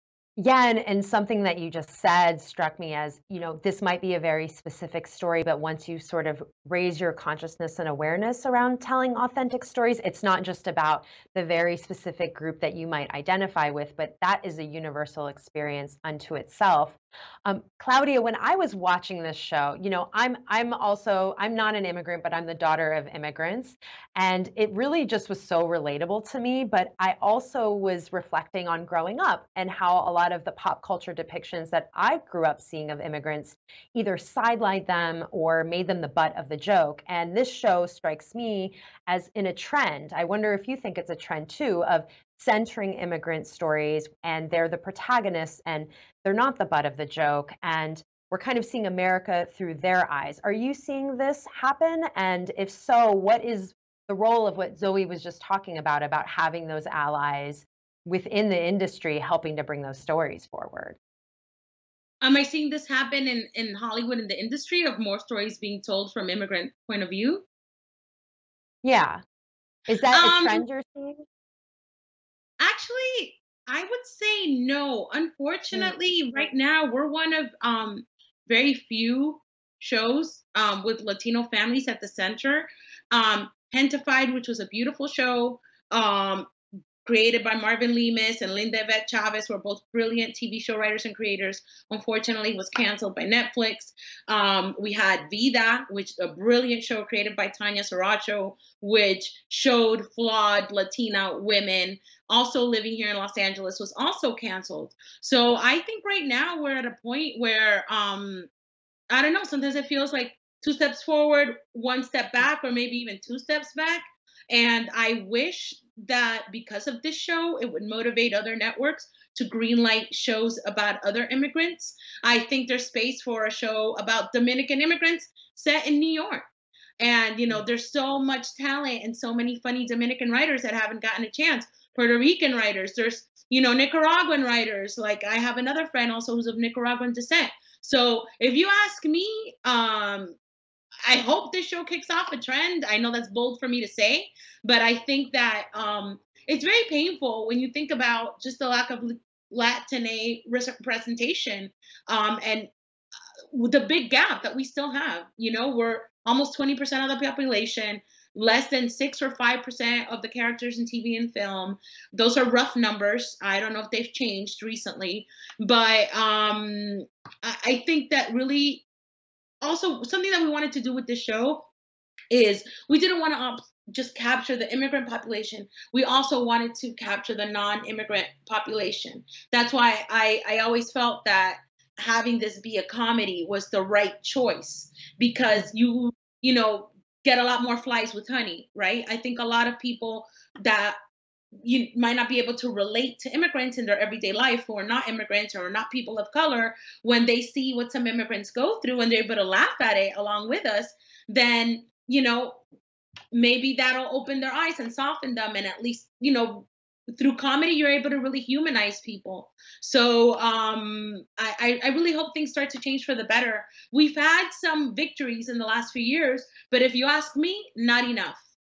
Forestieri on Latino representation in television
“Right now, we’re one of very few shows with Latino families at the center. ‘Gentified,’ which was a beautiful show created by Marvin Lemus and Linda Yvette Chávez, unfortunately was canceled by Netflix. We had ‘Vida’ which was a brilliant show  created by Tanya Saracho which showed flawed Latina women also living here in Los Angeles was canceled. Right now we’re at a point where, I don’t know, sometimes it feels like two steps forward, one step back, or even two steps back.” – Claudia Forestieri